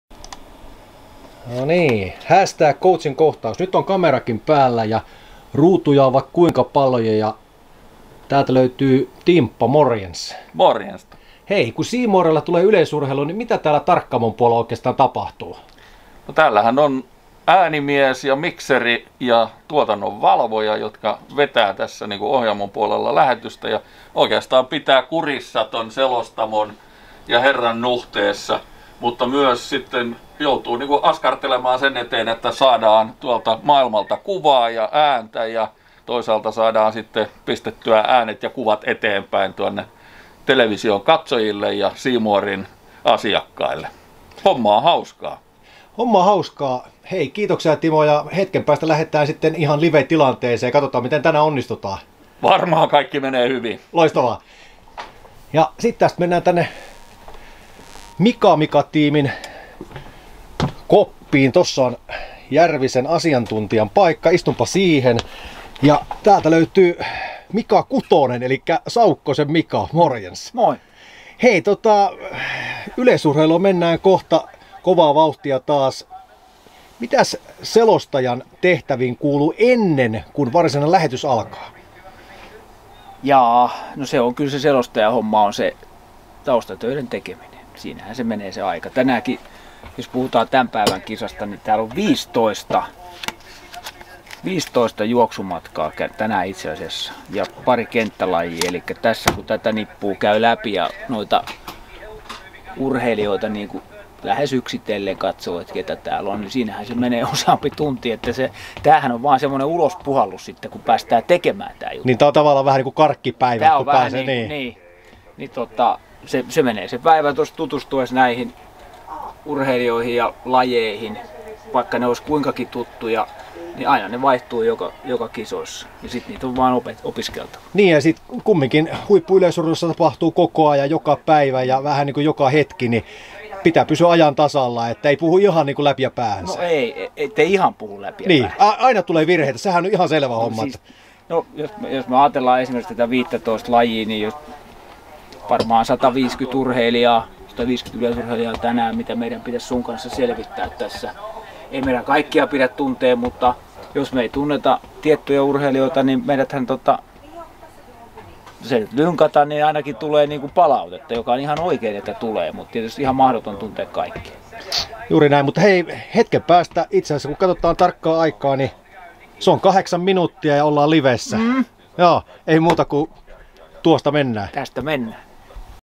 C-more sport - yu-kulissien takaa
Coach MJ áka C-moren yu:n asiantuntija kameran kanssa kulisseissa Pöllölaaksossa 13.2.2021 - kuvissa mukana Timo Haapaniemi ja Mika Saukkonen